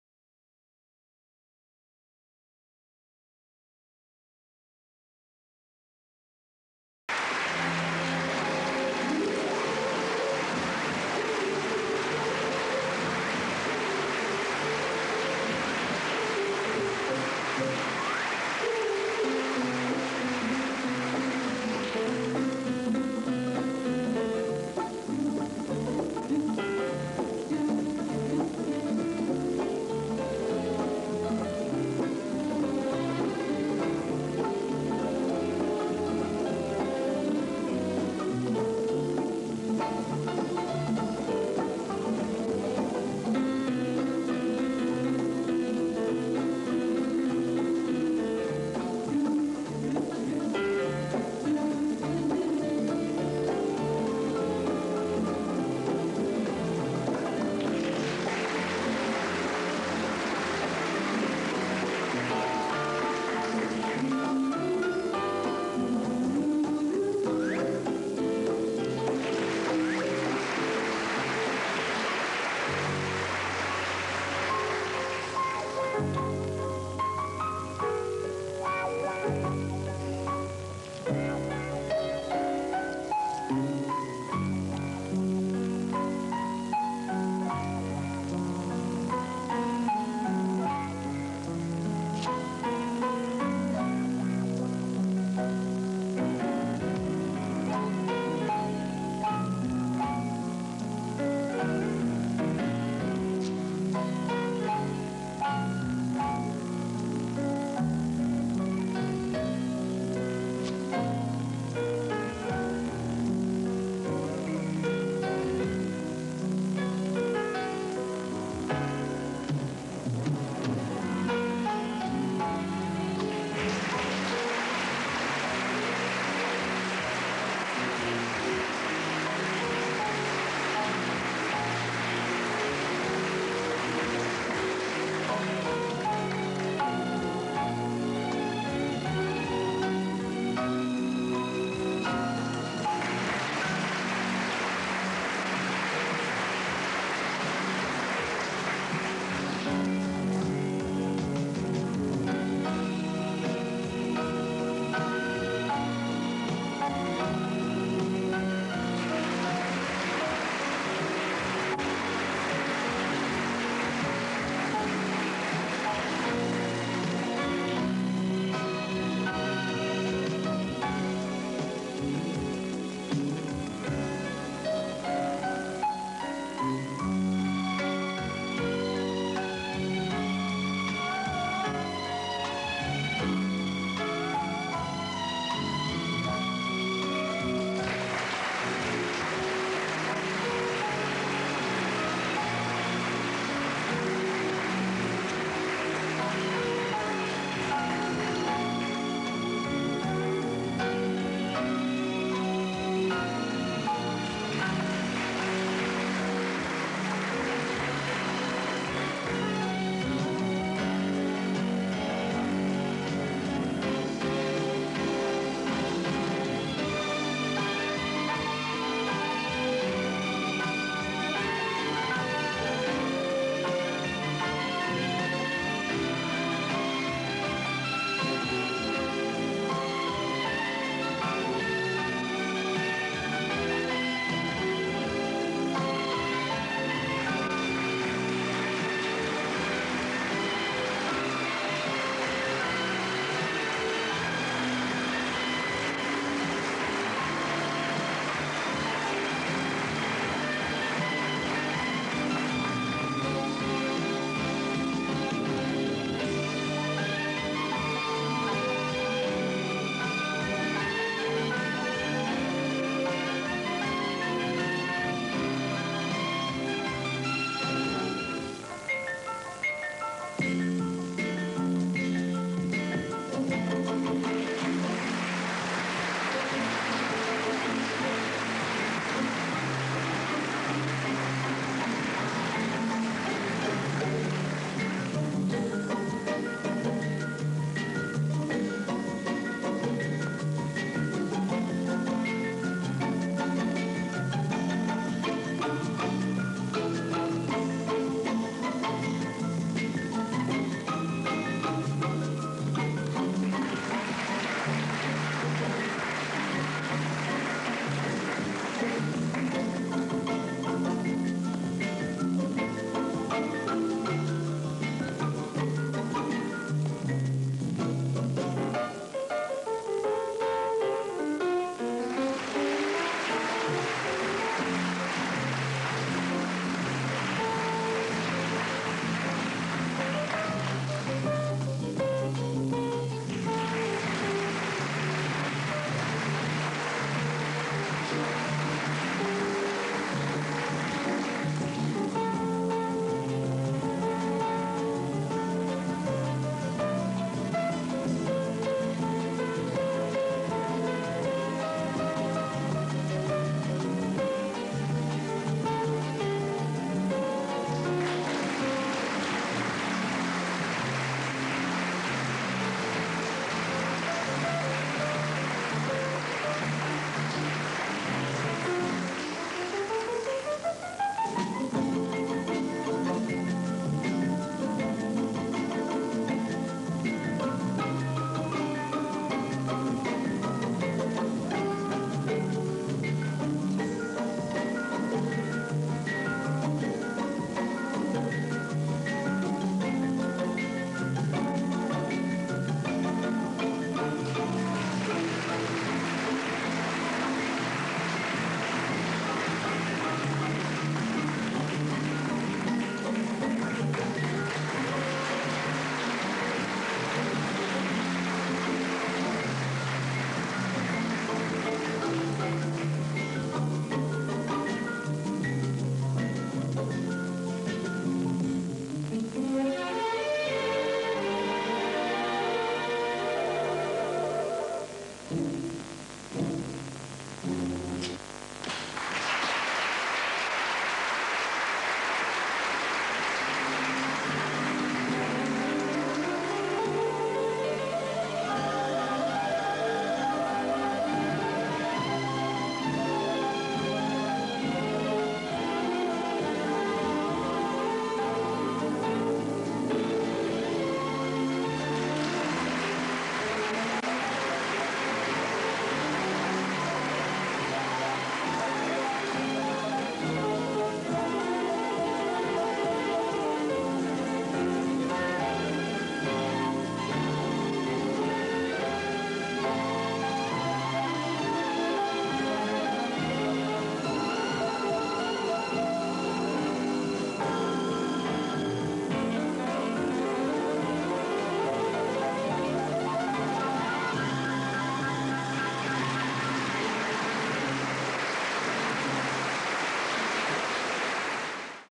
Fantasio, Canes & Candles, Deutsch TV - Mago Elite video collecation
Famous magician Fantasio performs his well known "canes and candles" act in a magic gala fot deutsche TV in the 80's
Il celebre mago Fantasio esegue il suo famoso numero con bastoni e candele in un gala magico, per la TV tedesca.